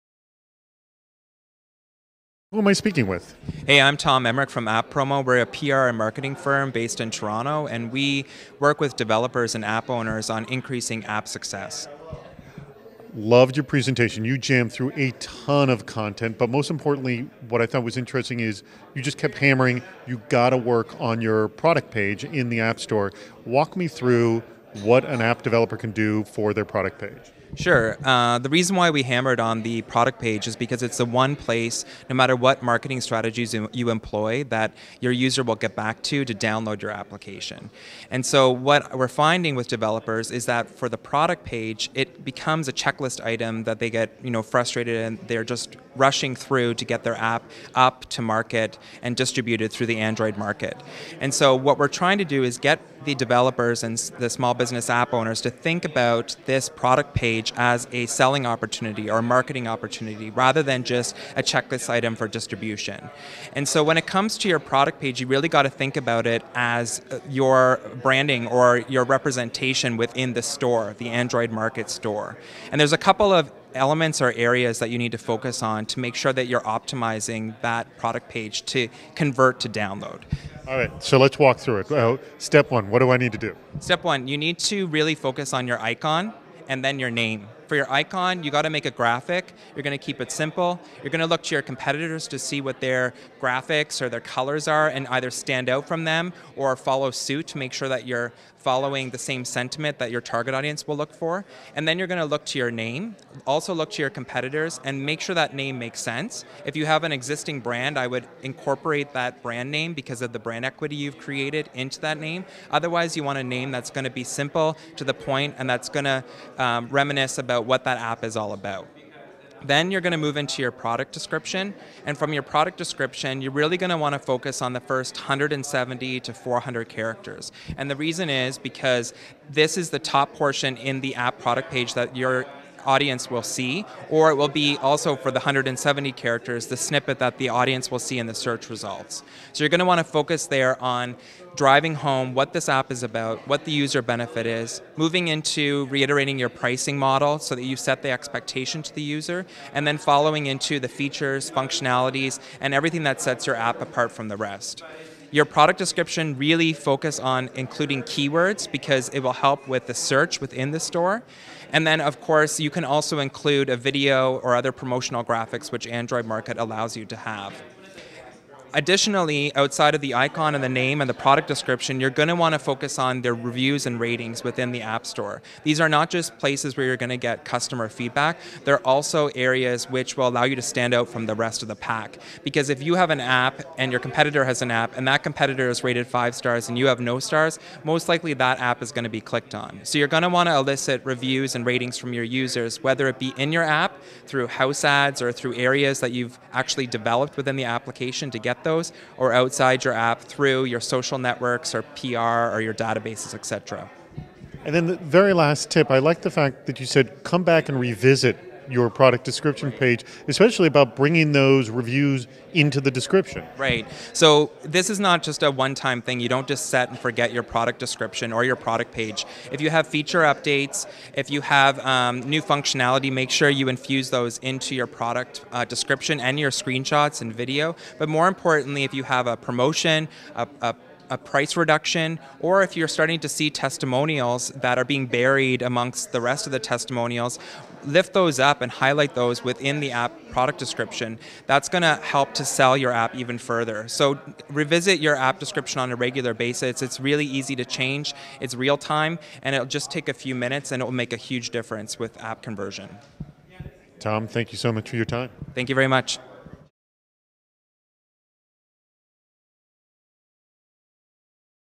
Tips to Enhancing Your App's Visibility in the Android Marketplace
The number one issue and concern of Android app developers is how do I get my app discovered and distributed. If your app is not one of the top 50 apps in the Android Marketplace, no one knows it exists.
At the Android Developers Conference, or AnDevCon, in Burlingame, California, Tom Emrich and his colleague Gary Yentin of App-Promo, a PR and marketing firm for apps, offered up some advice. The two gave a great presentation on how to increase discovery and distribution of your mobile app. They hammered through tons of tips, but the ones I thought were the most important were Emrich's advice on focusing on your product page in the Android Marketplace. Because no matter where consumers discover your app, everyone is going to always end up on your product page, said Emrich. That is the very last step before install. Depending on how you organize it, it will or won't sell your app.
Here's a summary of some of Emrich's advice on how to make the most of your product page in the Android Marketplace:
Focus on your icon and name: These are critical to branding. Look at your competitors. Differentiate your look and name, but you want a name that makes sense to what your app is.
Put your most important marketing text in the very beginning: The first 170 characters is what shows up in search results and the first 400 characters is what appears above the fold (before users have to scroll to see more). Include in that opening what your app is about and what the benefit is to the user. This is your app's elevator pitch.
Max out your product description: While the first 400 characters are the most critical, you have up to 4,000 characters for more description. Max it out with great explanation and lots and lots of keywords to increase discovery.
Focus on reviews and ratings: Elicit reviews and ratings from users either through ads, social networks, or requests through the app. These are critical to selling your app.
Reiterate the price: Don't lead your users on with a free app that's crippled until you pay up. Such hidden "gotcha" sales tactics will result in very low reviews and low star ratings which will hurt your app overall. If it's a freemium app, explain at what point they'll be paying. If it's truly free, explain that. If there's a price change, indicate that.
Make announcements: Use your product page to announce feature changes and updates. As you learn more about keywords, modify the description with those new keywords.
Republish testimonials: If your best testimonials are getting buried, republish them in your product description.
Revisit your page: This is not a set it and forget it piece of marketing collateral. You need to revisit it often and make sure it always looks fresh.
Include video: You can add a link to a YouTube demo video on your product page. No better way to demonstrate your app than with a great sample video.